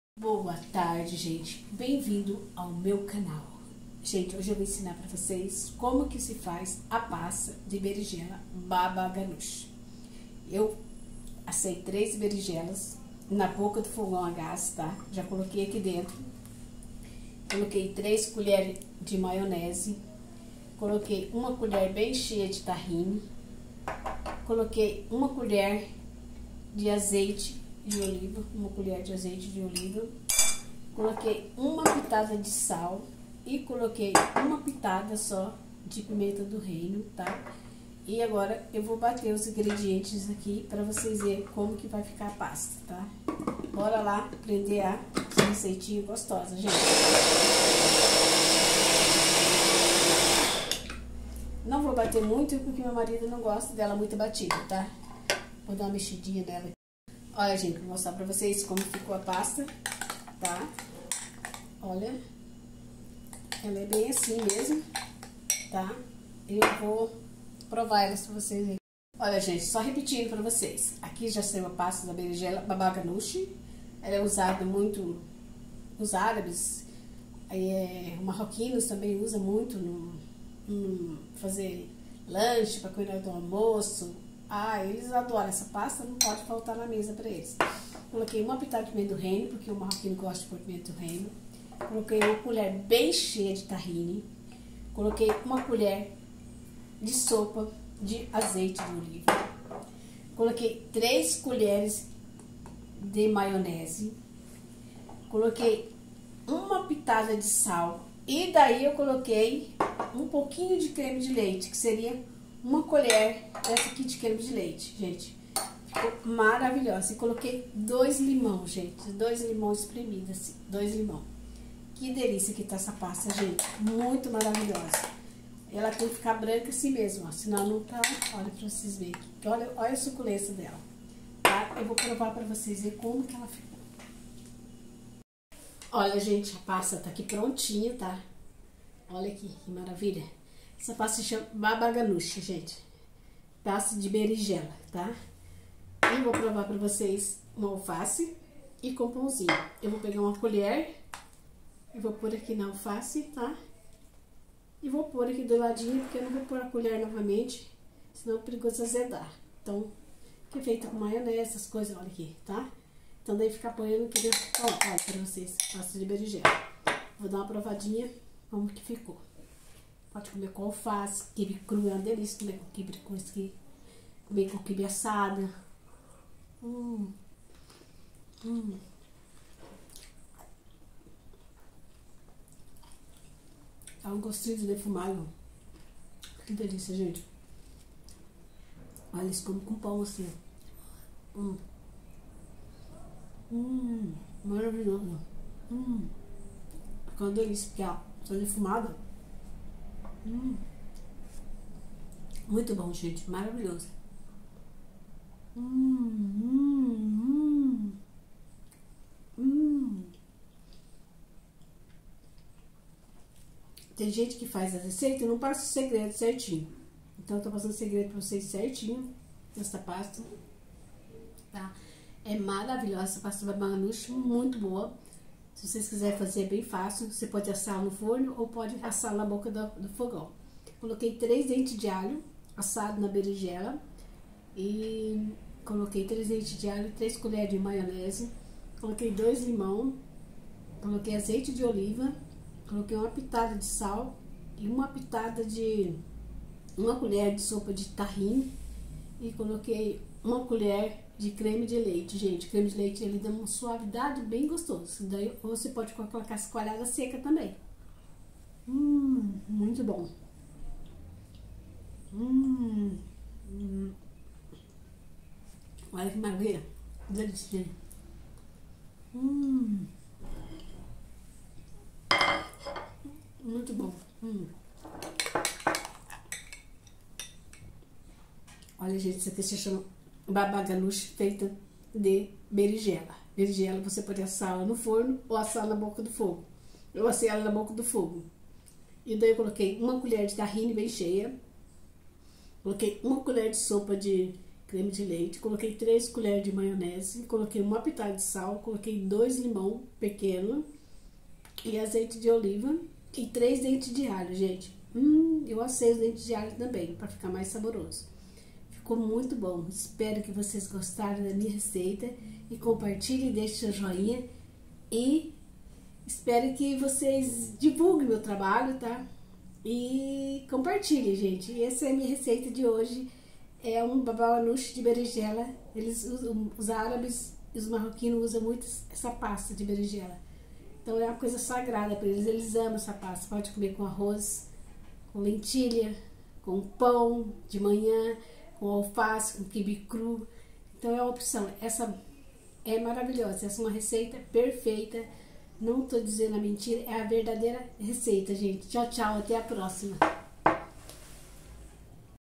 receita pasta de babaganuche uma delícia pra comer com salada e pão, kibe crú e feito berinjela 🍆🍆🍆.
pasta de babaganuche (pasta berinjela)
verdadeira receita árabe.